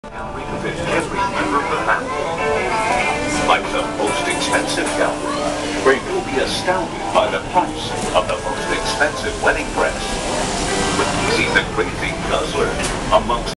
The Worlds Smallest Car!
While I was on vacation, they had a Guinness World Record center. The car is called the Peel P50. It is the smallest production car that is road legal.